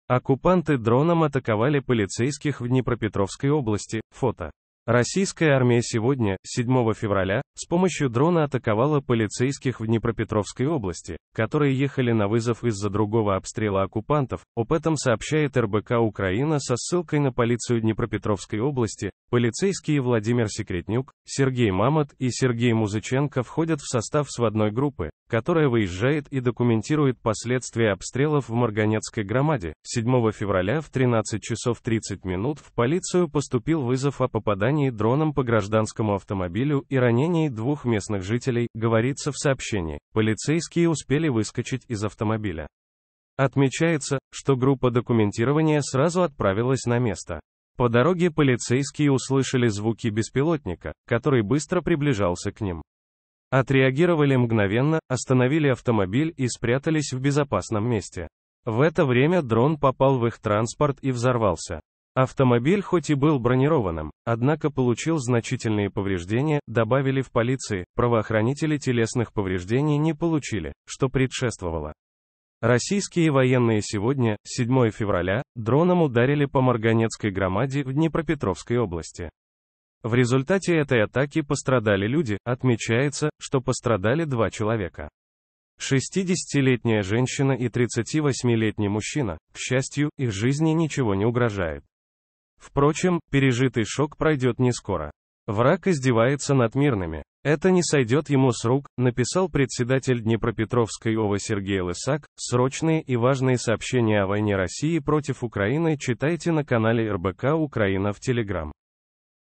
Оккупанты дроном атаковали полицейских в Днепропетровской области (фото).
РоссийскаяФедерация Днепропетровскаяобласть ВойнавУкраине Российская армия сегодня, 7 февраля, с помощью дрона атаковала полицейских в Днепропетровской области, которые ехали на вызов из-за другого обстрела оккупантов. Об этом сообщает РБК-Украина со ссылкой на полицию Днепропетровской области. Полицейские Владимир Секретнюк, Сергей Мамот и Сергей Музыченко входят в состав сводной группы, которая выезжает и документирует последствия обстрелов в Марганецкой громаде. "7 февраля в 13:30 в полицию поступил вызов о попадании дроном по гражданскому автомобилю и